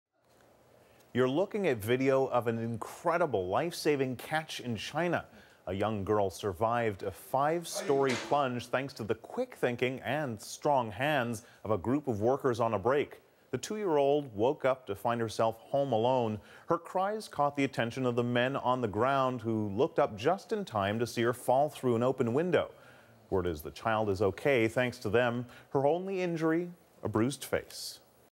Workers Catch Falling Toddler
A group of workers on a break managed to catch a two year-old girl who fell from fifth floor apartment window.